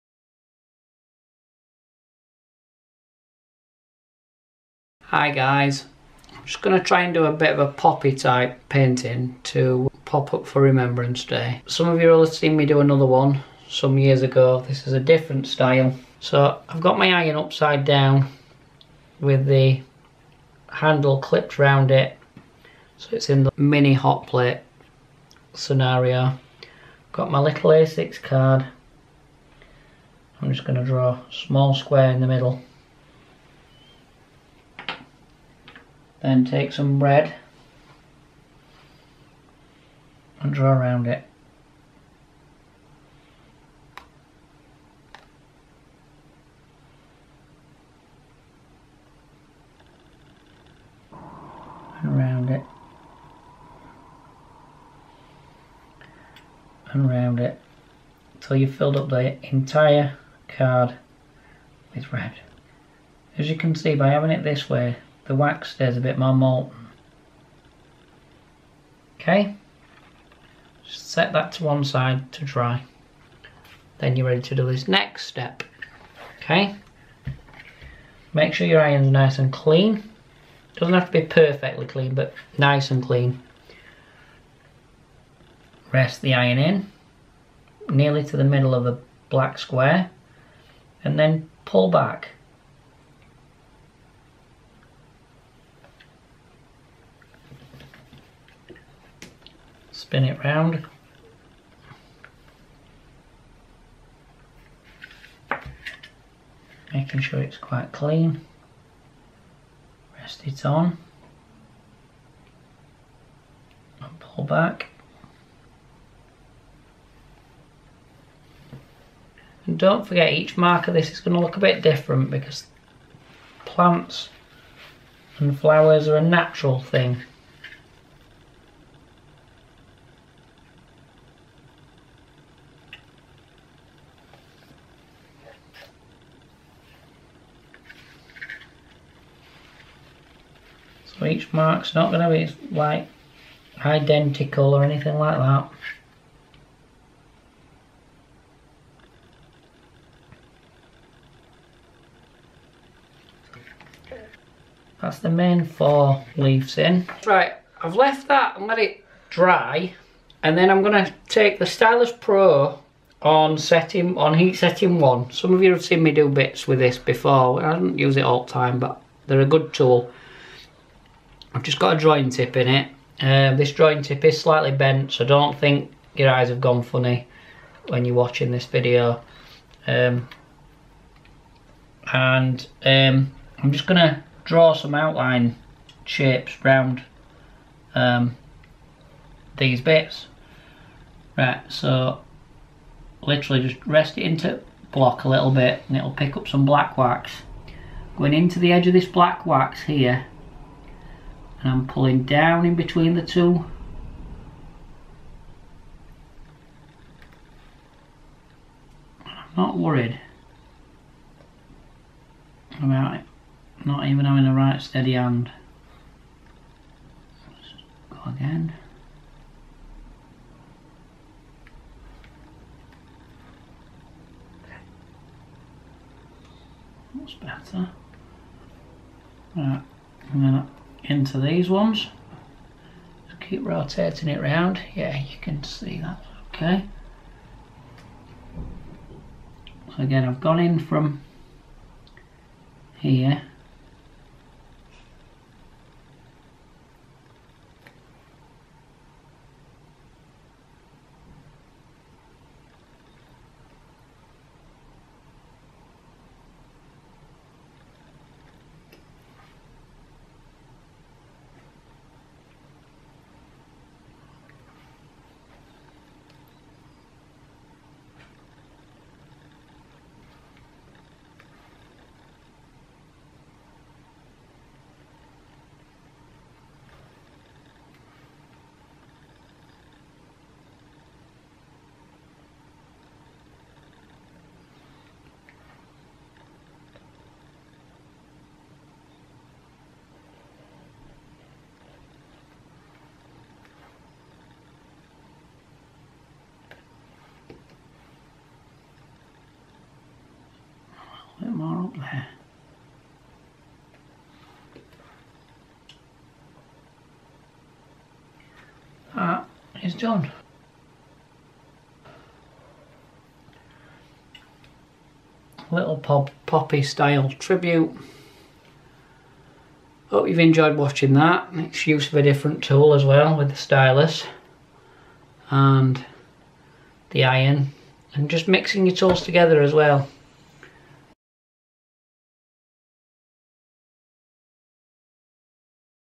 Remembering the Fallen with Encaustic Art on Armistice Day
2023 Armistice Day, we remember those who fell fighting for our freedom in this Armistice Day special featuring the painting of a Poppy using both the Encaustic Art Painting Iron and the Encaustic Art Stylus Pro Tool